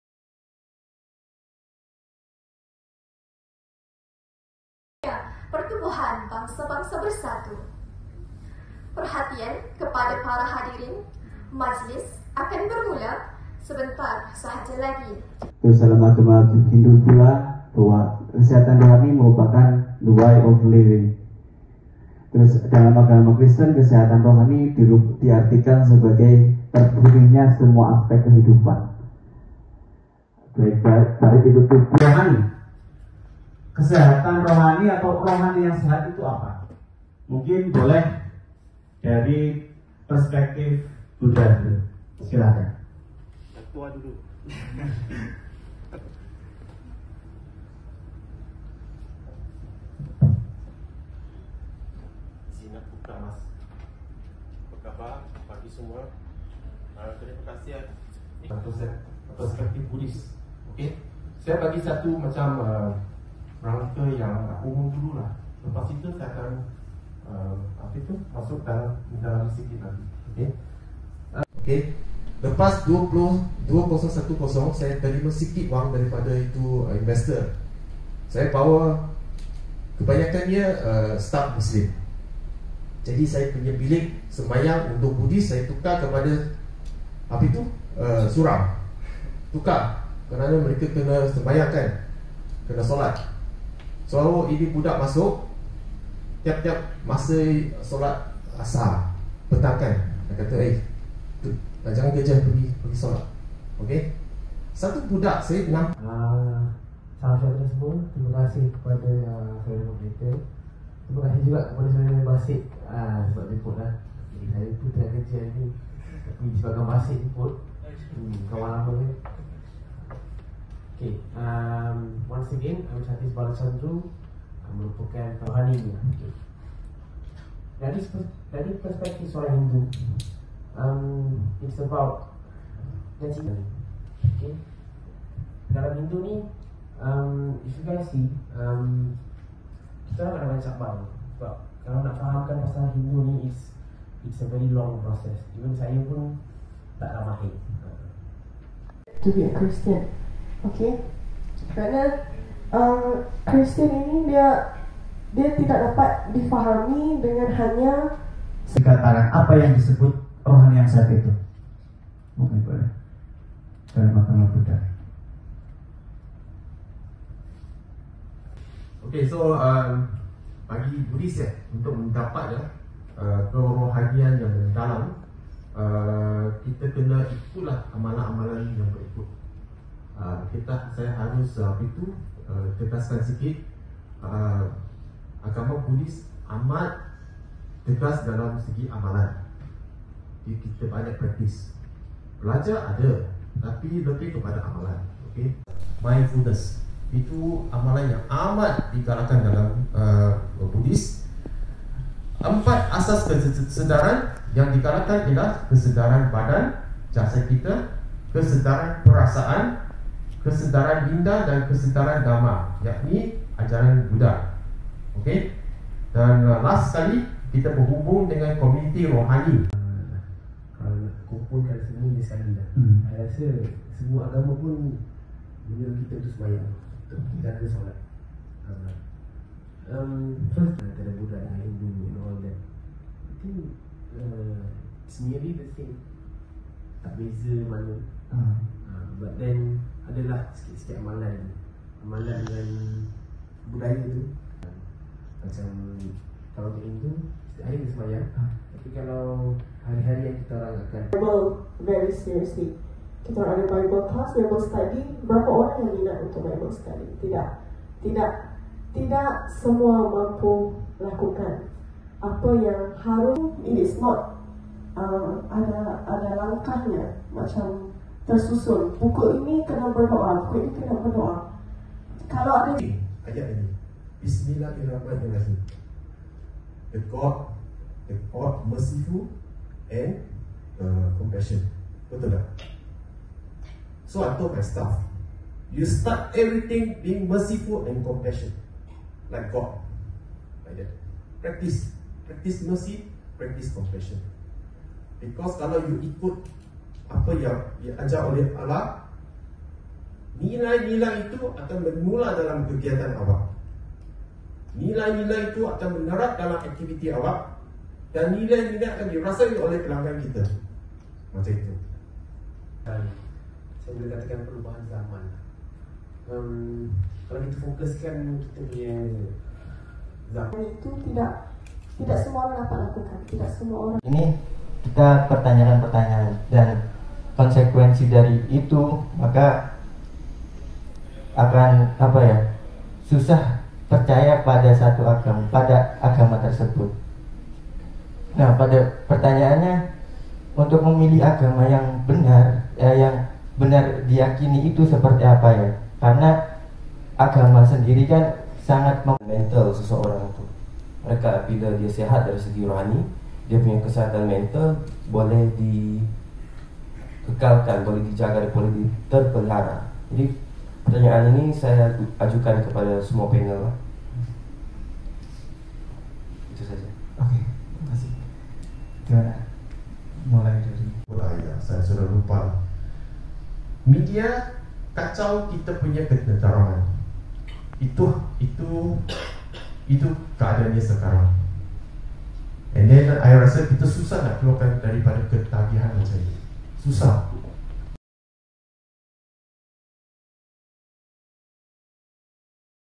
Forum on Spiritual Health in the Perspective of Religions
Forum on Spiritual Health in the Perspective of Religions in Conjunction with the Celebration of United Nations World Interfaith Harmony Week
Organiser: Course PPPH3623 Comparative Mysticism, Universiti Kebangsaan Malaysia (UKM)